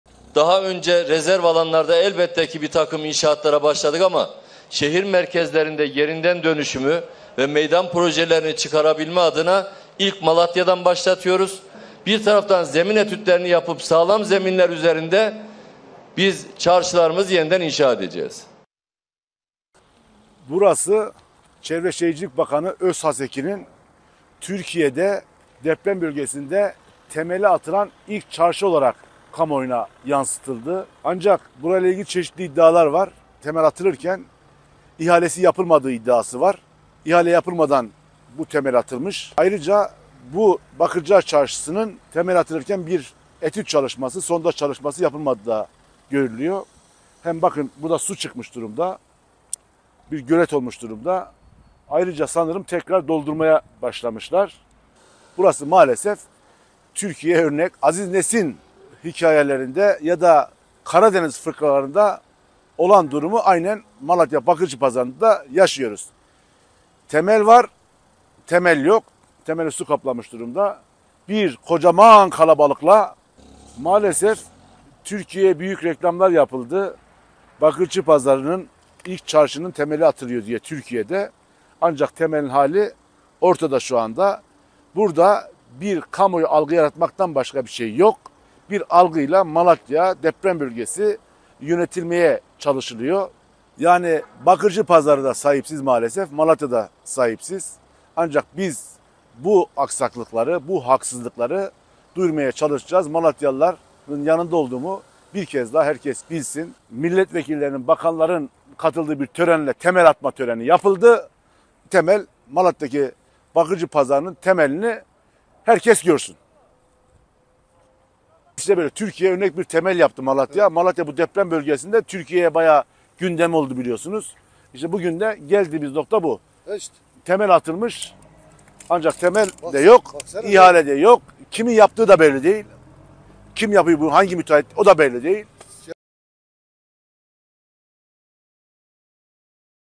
Bakan Özhaseki'nin açtığı çarşıyı su bastı! CHP'li Ağbaba ifşa etti: "İhalesiz temel atılmış!"
CHP Malatya Milletvekili Veli Ağbaba, Çevre, Şehircilik ve İklim Değişikliği Bakanı Mehmet Özhaseki’nin 29 Temmuz’da açılışını yaptığı deprem bölgesi Malatya’daki Bakırcılar Çarşısı inşaatının zeminini su bastığını belirterek, “Temel atılırken ihalesi yapılmadığı iddiası var, ihale yapılmadan bu temel atılmış. Ayrıca bu ‘Bakırcılar Çarşısı’nın temeli atılırken bir etüt, sondaj çalışması yapılmadığı görülüyor… Algıyla Malatya deprem bölgesi yönetilmeye çalışılıyor” dedi.